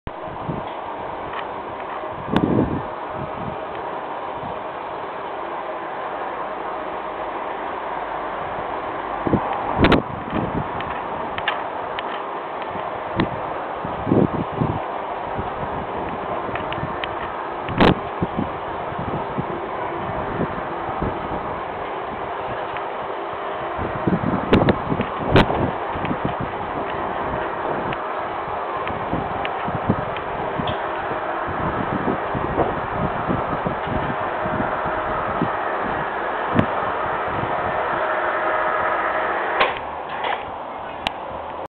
3 x Patio Awnings .mov
3 no Electric Awnings installed at a Nursery School to protect the children from Rain ans Sun. Sized between 5.5 mts and 6mts in width x 4mts projection. All powered by Somfy tubular motors with remote hand sets and a wind sensor for safety. These awnings are Toga semi cassettes with a carry bar system by Voss. We also supply awnings from all the major manufactureres including Arquati and Weinor.